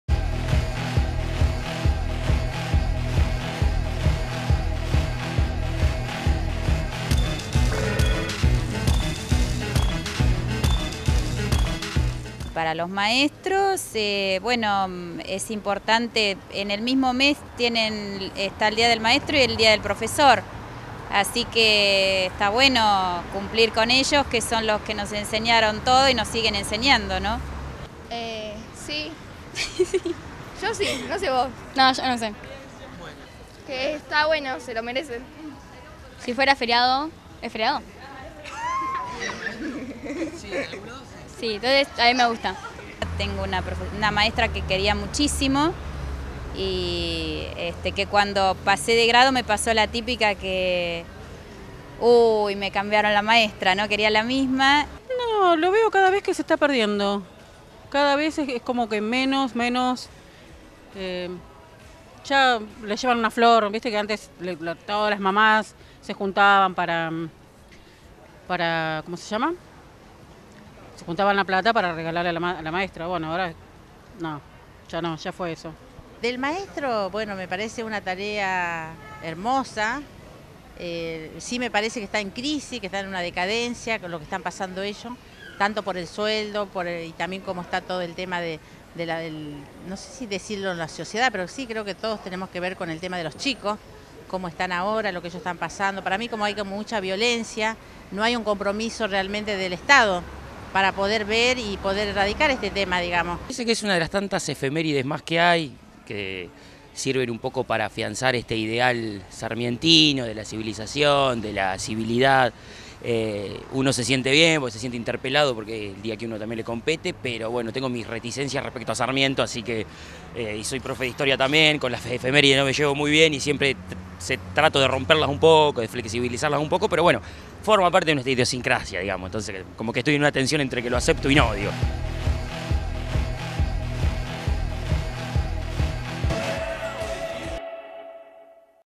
EncuestasTVU | ¿Qué onda el día del maestro?
Salimos por las calles de la ciudad a preguntar: ¿Qué onda el día del maestro? y vos ¿qué opinas?